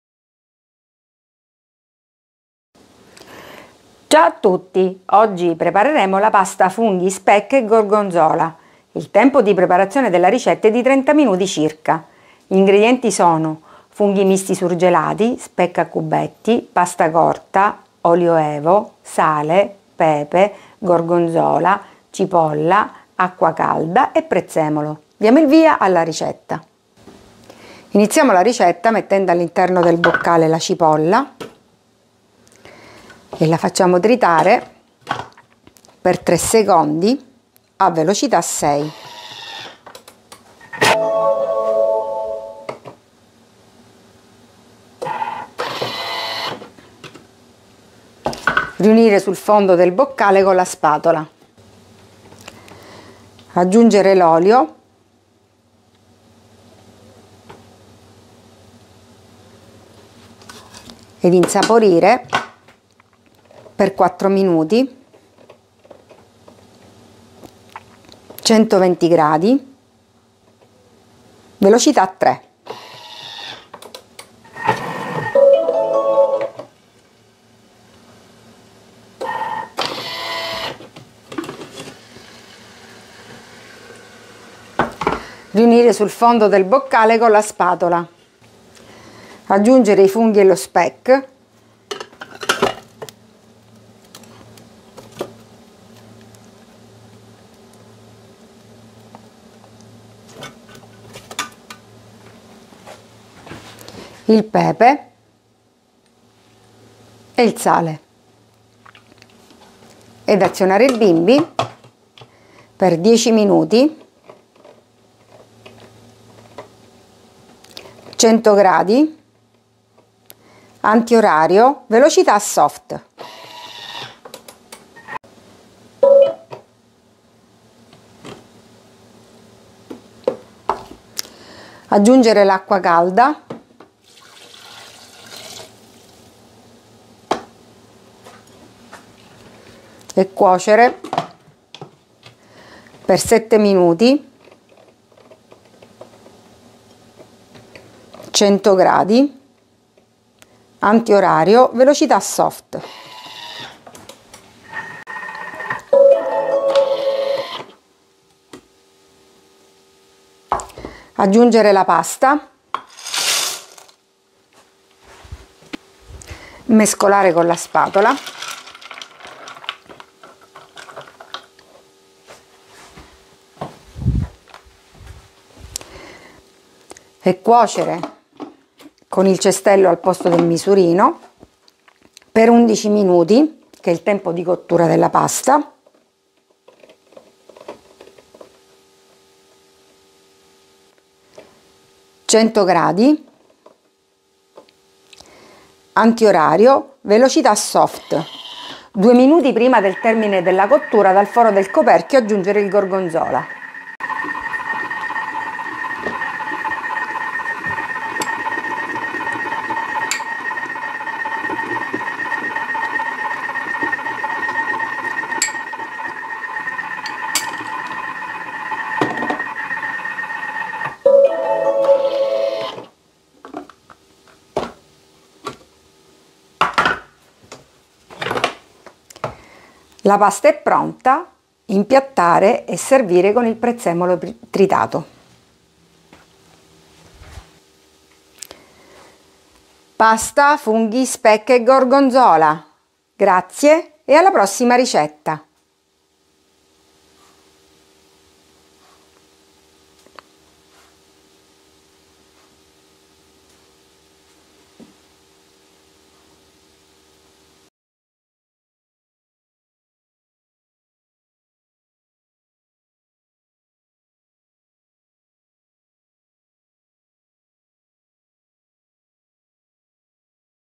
Pasta funghi speck e gorgonzola per bimby TM6 TM5 TM31
Ingredienti:
350 g di funghi misti surgelati
100 g di speck tagliato a cubetti
320 g di pasta corta
30 g di olio e.v.o.
sale e pepe q.b.
100 g di gorgonzola dolce o piccante
30 g di cipolla
500 g di acqua calda
prezzemolo tritato q.b.

English
ingredients
350 g of frozen mixed mushrooms
100 g of speck, diced
320 g of short pasta
30 g of extra virgin olive oil
salt and pepper to taste
100 g of sweet or spicy gorgonzola
30 g of onion
500 g of hot water
chopped parsley to taste

Español
ingredientes
350 g de champiñones mixtos congelados
100 g de speck, cortado en cubitos
320 g de pasta corta
30 g de aceite de oliva virgen extra
sal y pimienta al gusto
100 g de gorgonzola dulce o picante
30 g de cebolla
500 g de agua caliente
perejil picado al gusto

Deutsch
zutaten
350 g gefrorene gemischte Pilze
100 g Speck, gewürfelt
320 g kurze Nudeln
30 g extra natives Olivenöl
Salz und Pfeffer nach Geschmack
100 g süßer oder würziger Gorgonzola
30 g Zwiebel
500 g heißes Wasser
gehackte Petersilie nach Geschmack